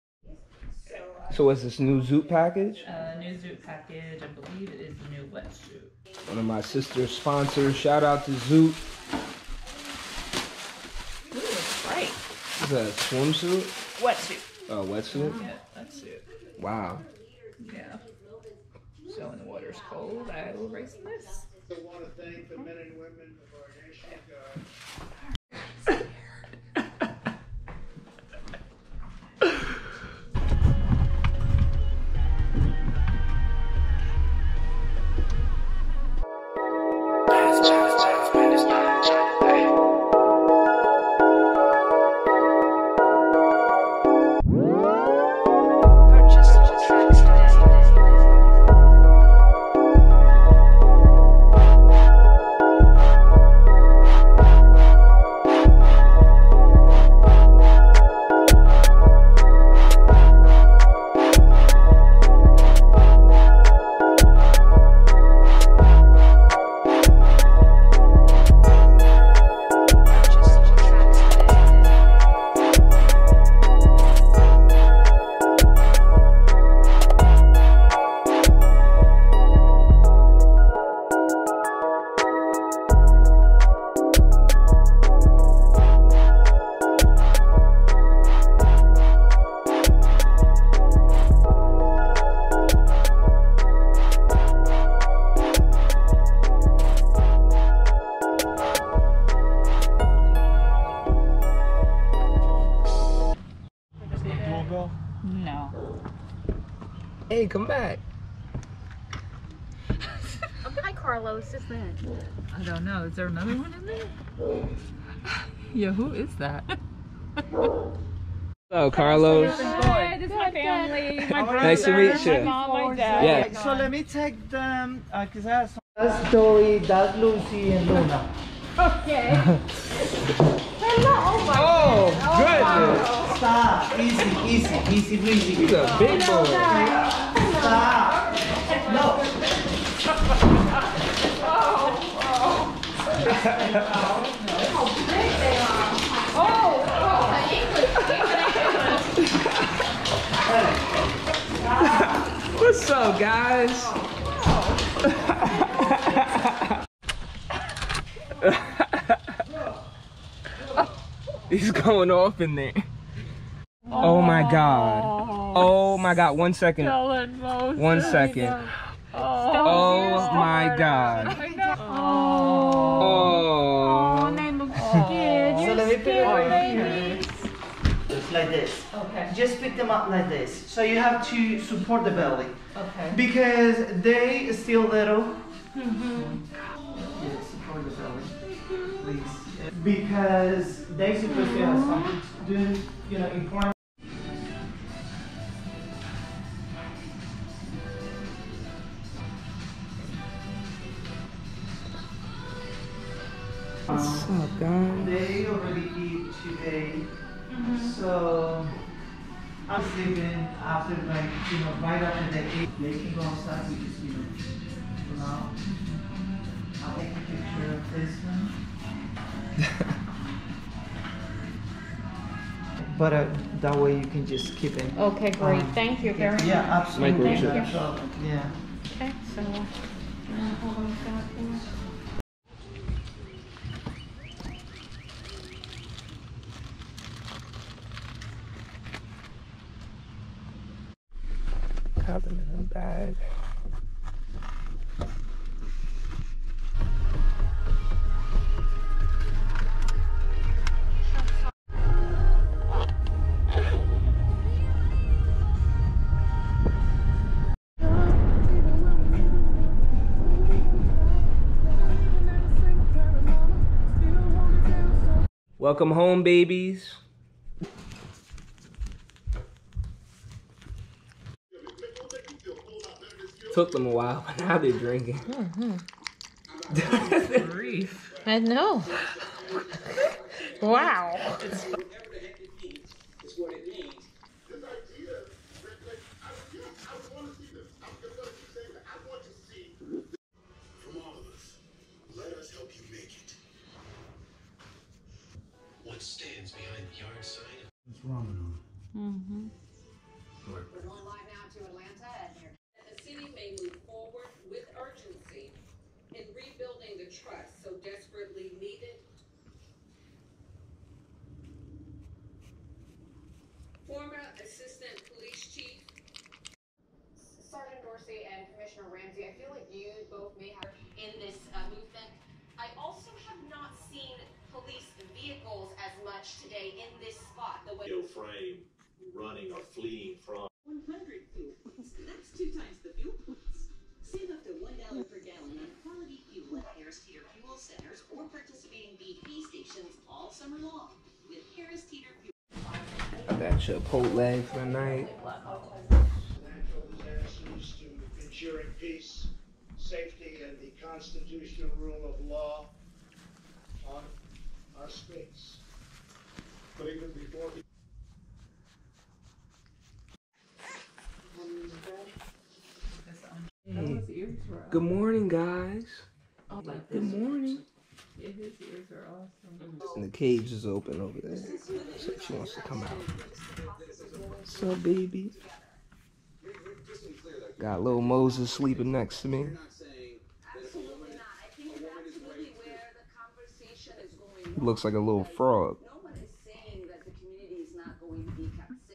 PICKING UP MY TWO NEW FRENCH BULLDOG PUPPIES (VLOG)
Welcome to the Family Stella and Moses! Two Beautiful Blue French Bulldog Puppies. Hope You Guy Enjoyed This Vlog.